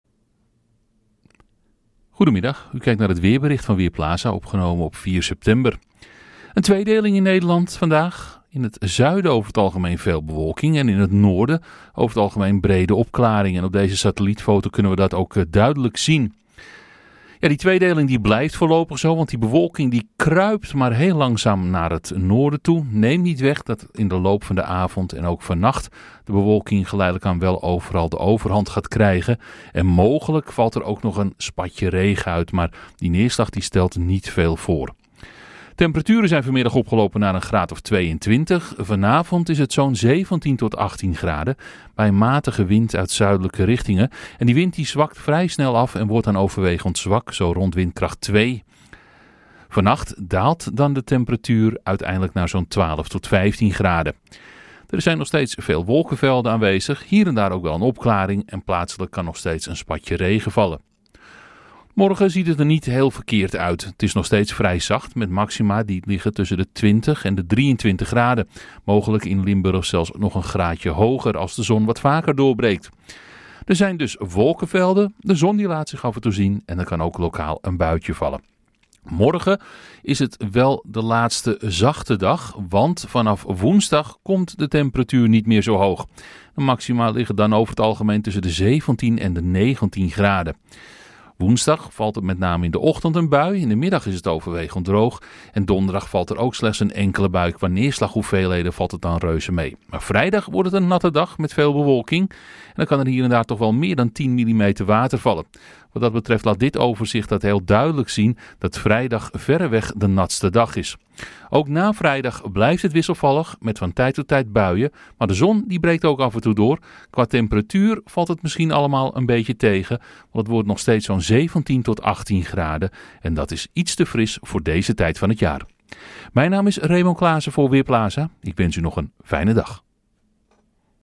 Weerbericht maandagmiddag: Nog even zacht
Vandaag en morgen zien we nog twintigers op de thermometer. Daarna wordt het wisselvalliger. Frisser, buien en meer wind.